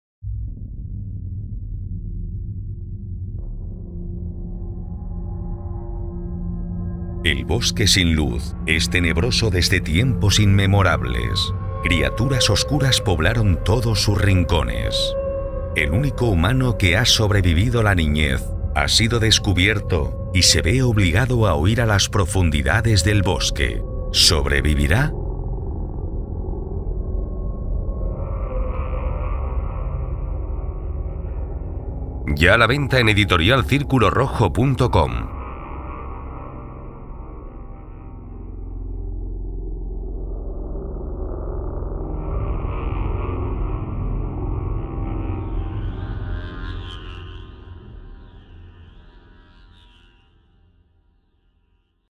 El Bosque Sin Luz (Booktrailer) - Editorial Círculo Rojo
El Bosque Sin Luz guarda muchos secretos. Es tenebroso desde tiempos inmemoriales, lleno de criaturas oscuras y poderosas.
En el poblado de los duendes existen niños, aunque nadie sabe de dónde vienen, y ninguno de ellos puede sobrevivir más allá de los siete años… El resto de la raza humana fue exterminada. Todos excepto uno. El elegido ha sobrevivido y todos han depositado en él la esperanza de devolver la luz al Bosque.
Su propia curiosidad por el mundo exterior hace que sea descubierto. Ahora tiene que huir para salvarse… las tinieblas le esperan… El camino no se presenta fácil, la pregunta es… ¿será lo suficientemente fuerte para sobrevivir?